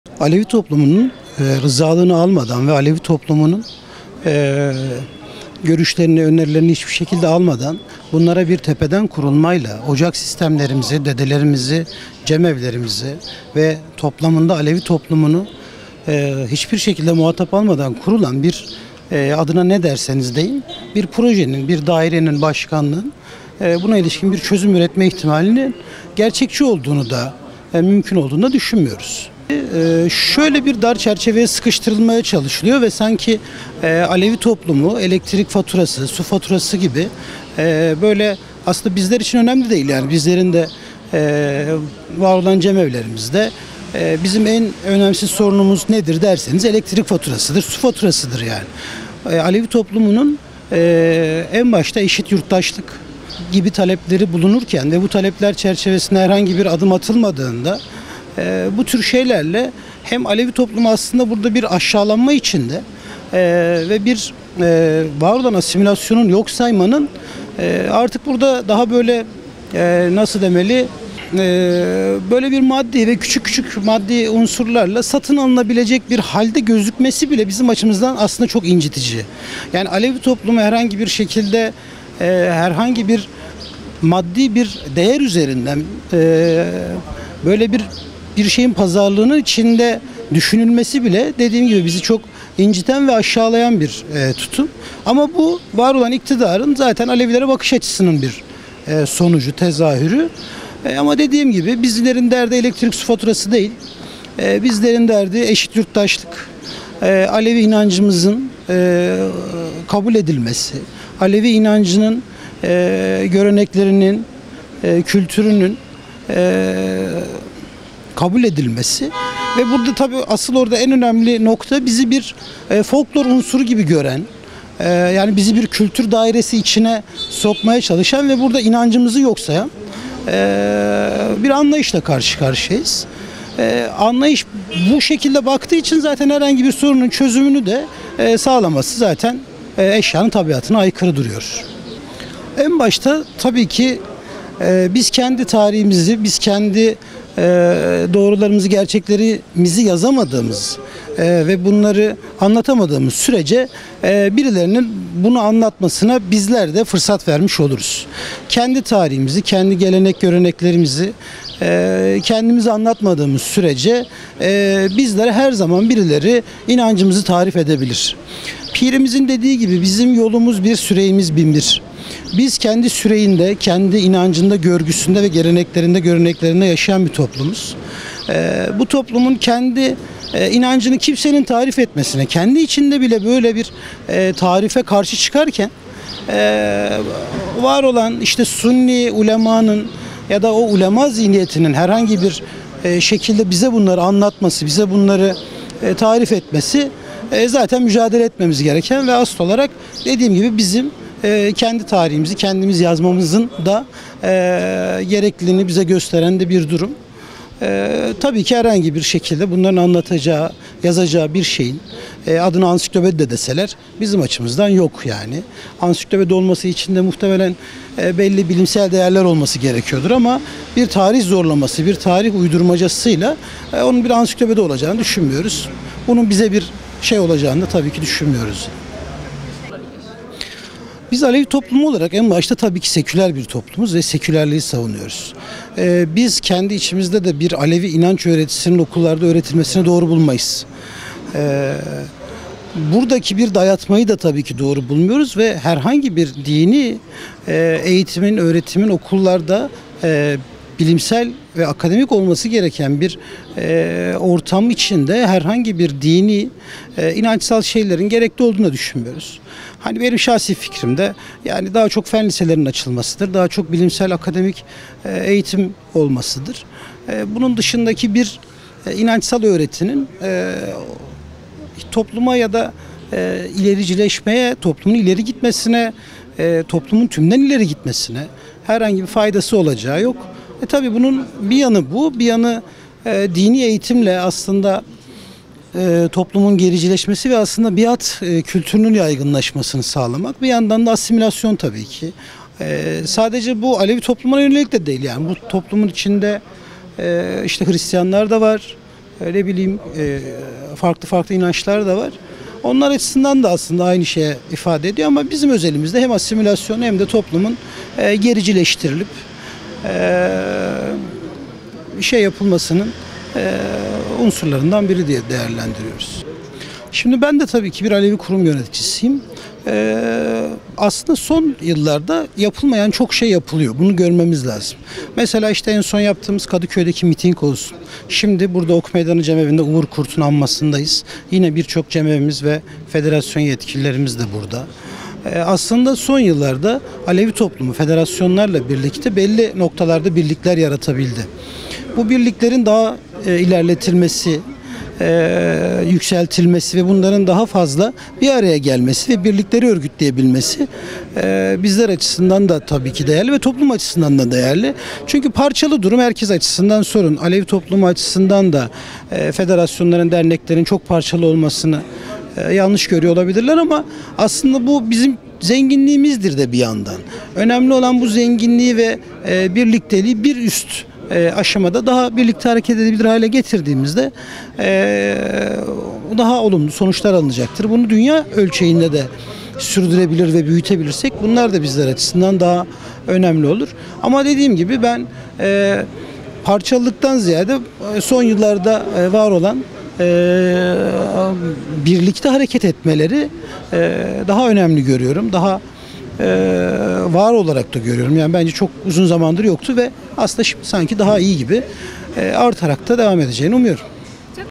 ‘Sünni ulema zihniyetinin inancımızı bize anlatması mücadele etmemiz gereken bir durum’
PİRHA- Alevi Bektaşi Kültür ve Cemevi Başkanlığı’nın hazırlatmak istediği Alevi Bektaşi Ansiklopedisi’nin ‘bir tarih uydurması’ olacağını ifade eden Esenler Cemevi Erhan Şahin, “Sünni ulema zihniyetinin herhangi bir şekilde inancımızı bize anlatması bizim mücadele etmemiz gereken bir durum. Adına ansiklopedi de deseler bizim açımızdan bir kabulü yok. Bir tarih uydurmacasıyla onun bir ansiklopedi olacağını düşünmüyoruz” dedi.